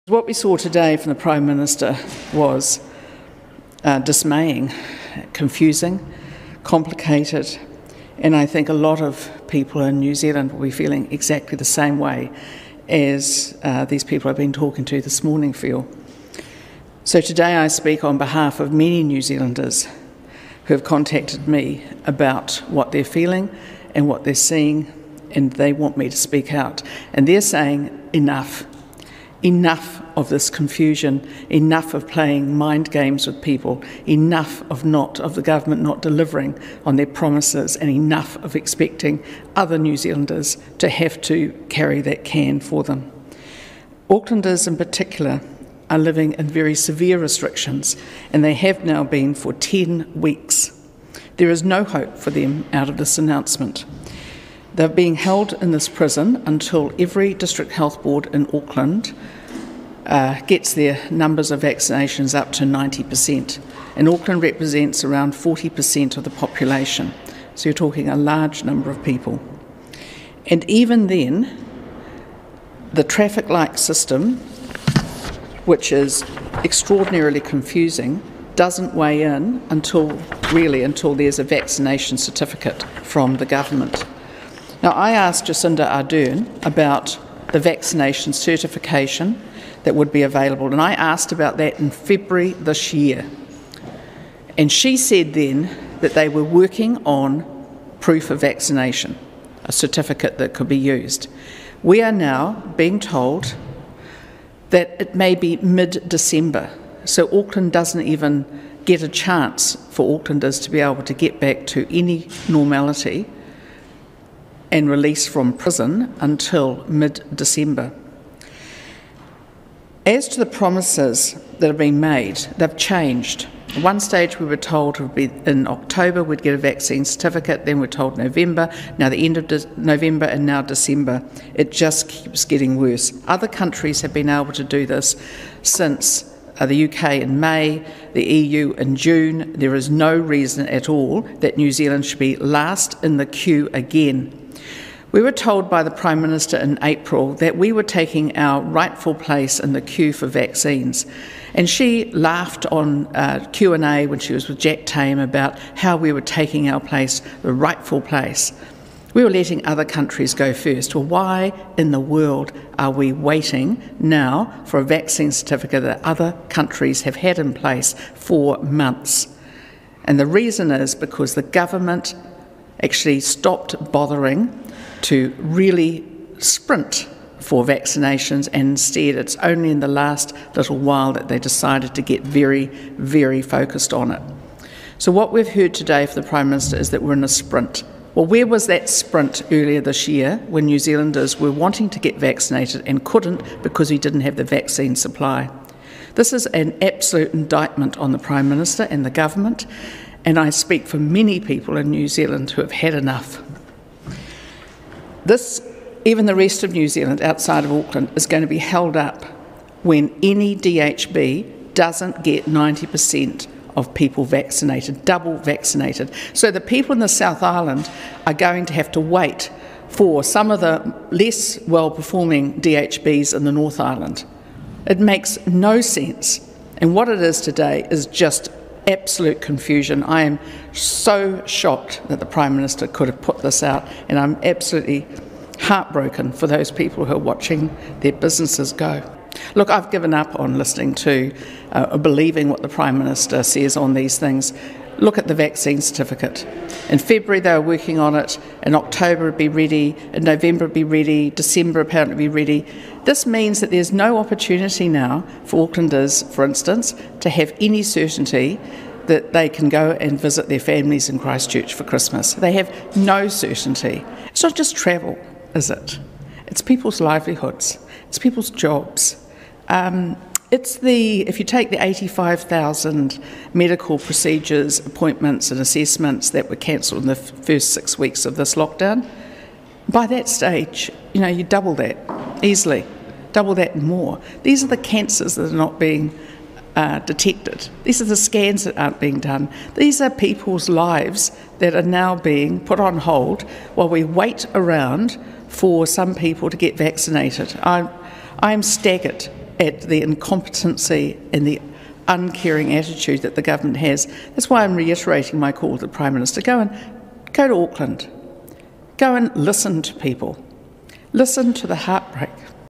'Listen to the heartbreak' - Judith Collins on the new Covid traffic light system
'Listen to the heartbreak' - National leader Judith Collins responds to PM Jacinda Ardern's announcement of a new traffic light system used to manage Covid-19.